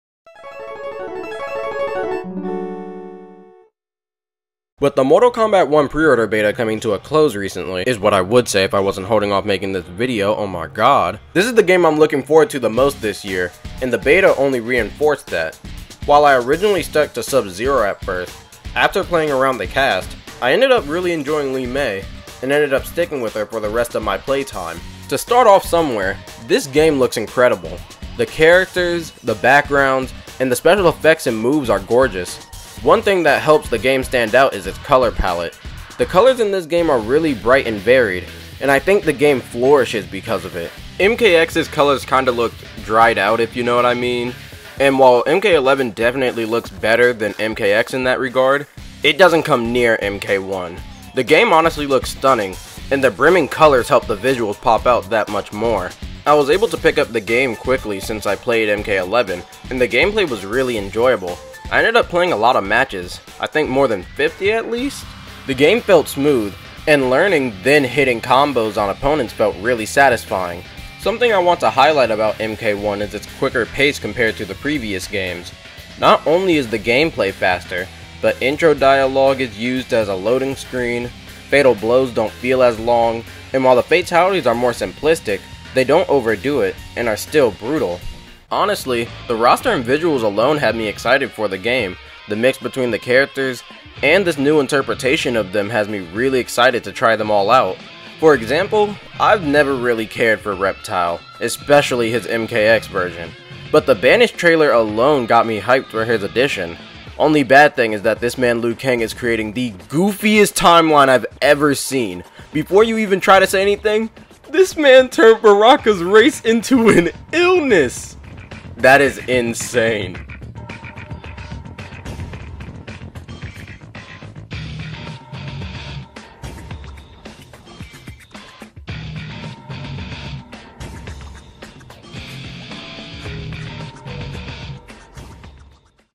My Thoughts On Mortal Kombat 1
With the not so recent closing of the MK1 Pre order beta, I wanted too get my thoughts about MK1 out there. I'm really excited for MK1, and plan to play the game on the channel.